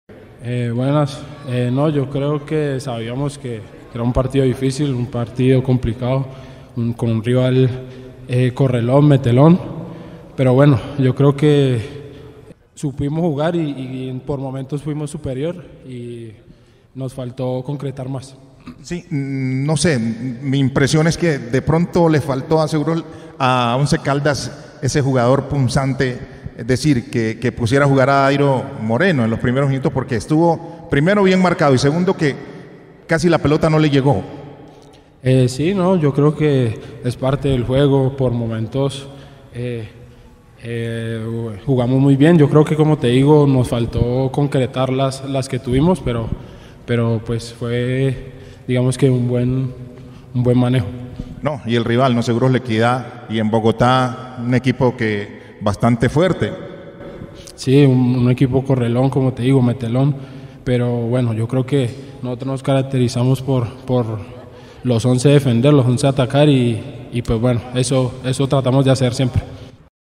Luis Palacios habló del empate ante La Equidad
El extremo de Once Caldas, Luis Palacios, también habló con "Las Voces" al final del partido ante Club Deportivo La Equidad Seguros ¡Escucha lo que dijo! ⚽️🏆